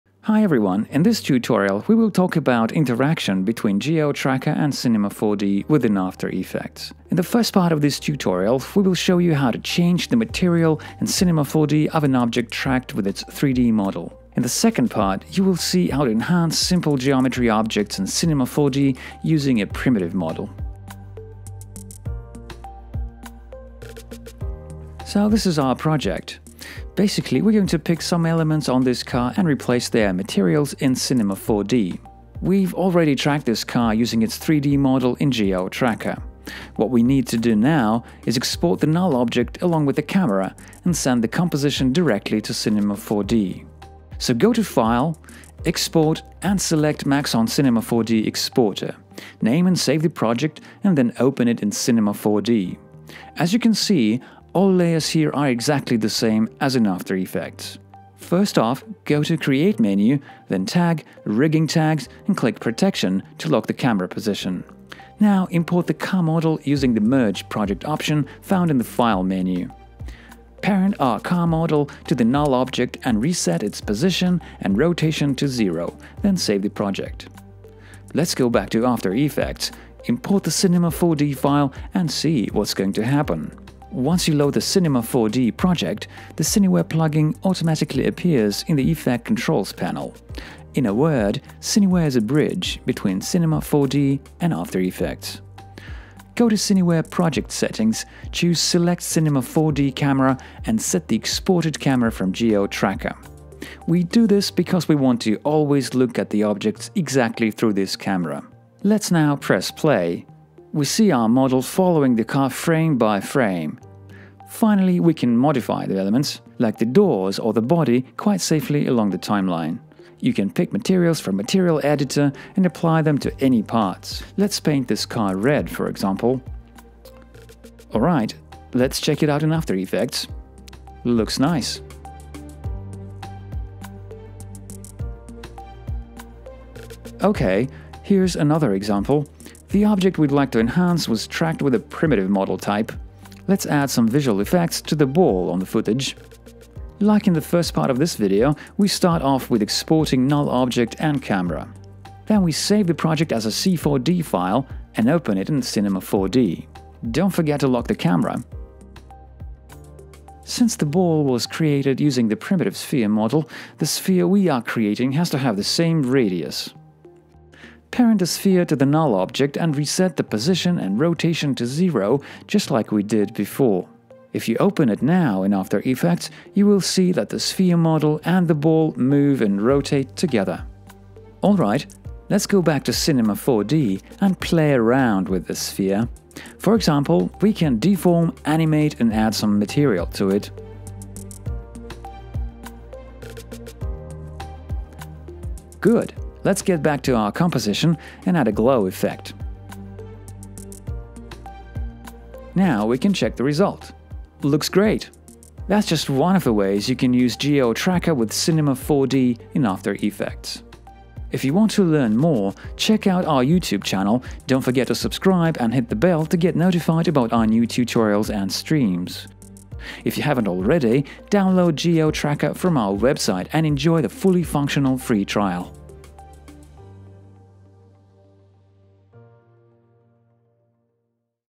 Integration with Cinema 4D – GeoTracker for After Effects Tutorial
In this video you will see how to pass tracking results from GeoTracker to Cinema 4D and then send them back to After Effects. Follow this step by step tutorial to learn how to enhance any 3D object tracked with GeoTracker in CInema 4D.

Timecodes:
0:00 –  Introduction
0:21 – Custom Model
2:13 – Primitive Model